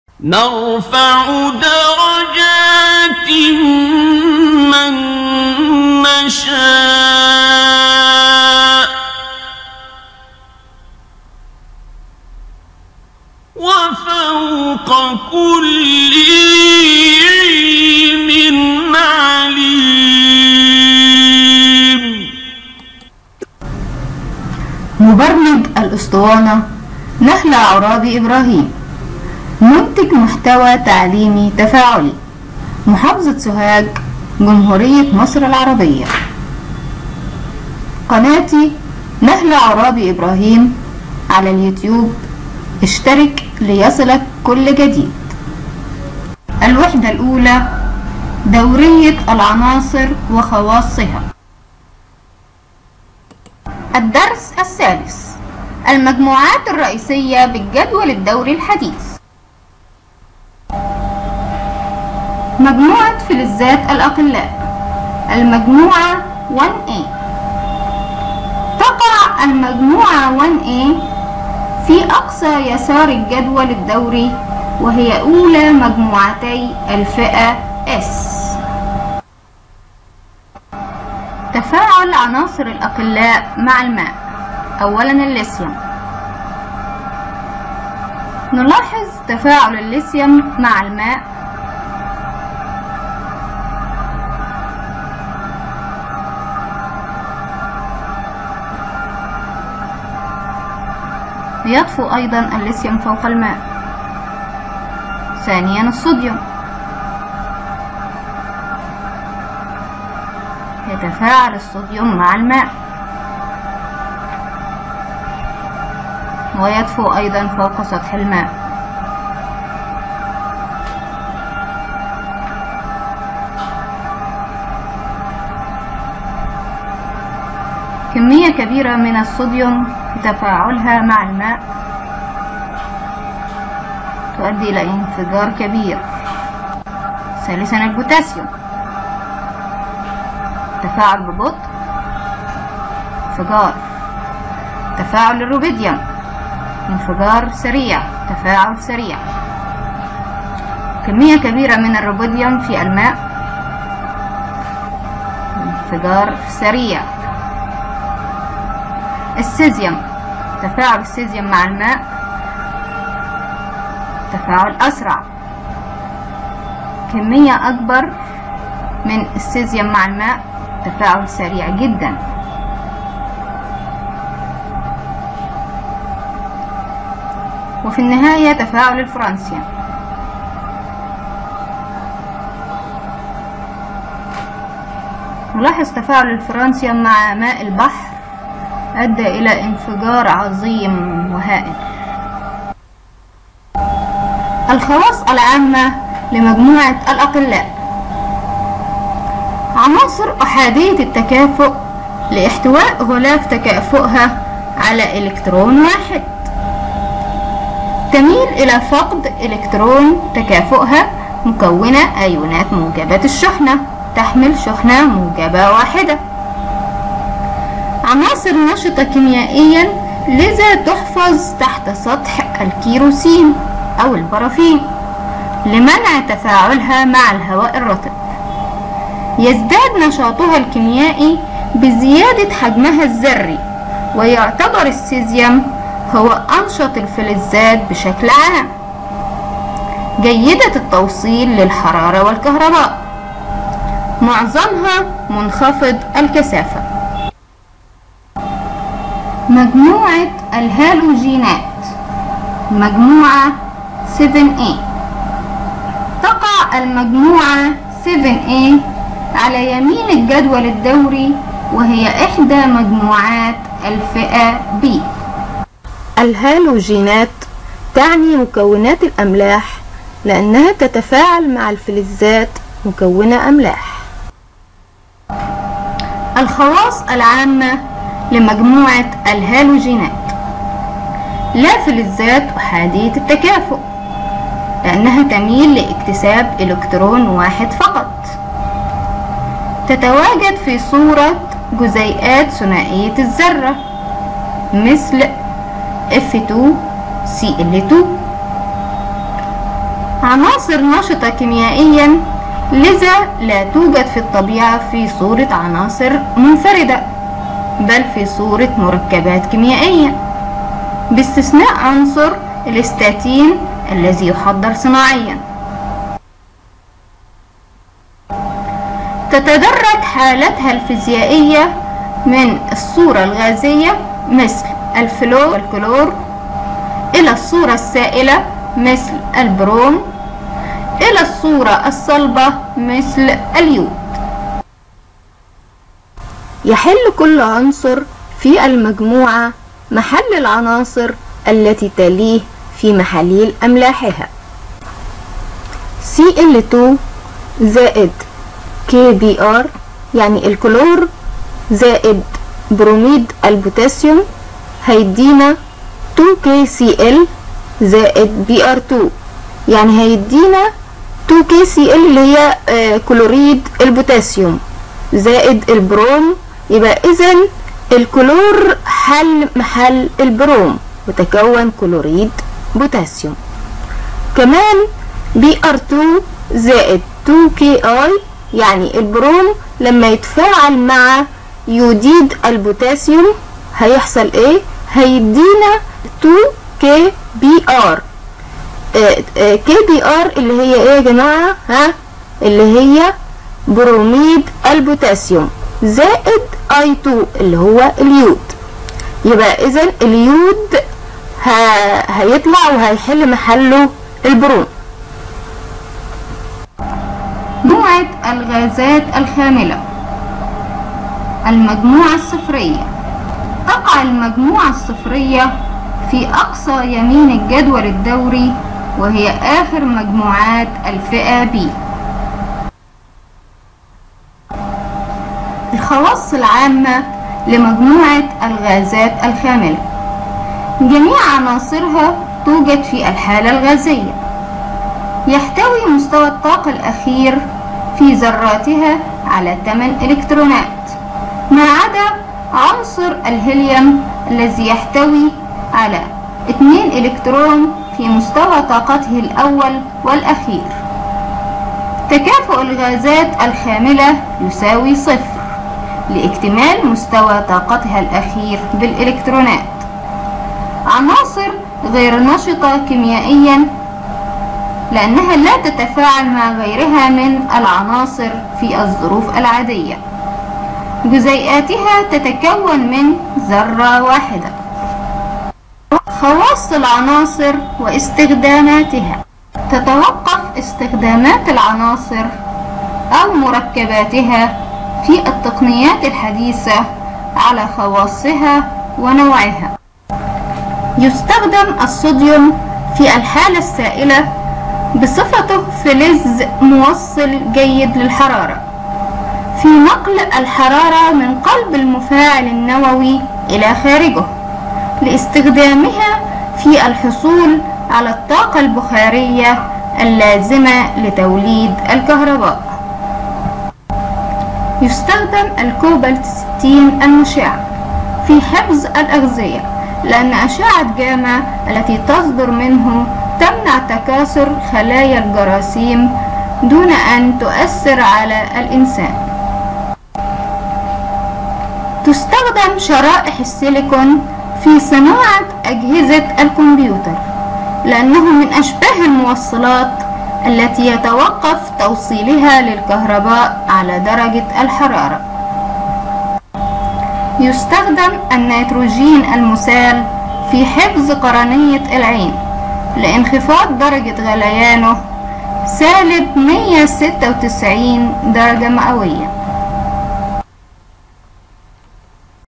المجموعات الرئيسية في الجدول الدوري الحديث للصف الثاني الإعدادي
اسطوانة تعليمية تفاعلية للدرس علوم الصف الثاني الإعدادي الفصل الدراسي الأول الوحدة الأولى
لتحميل الأسطوانة التفاعلية اضغط على الرابط ثم فك الضغط على جهازك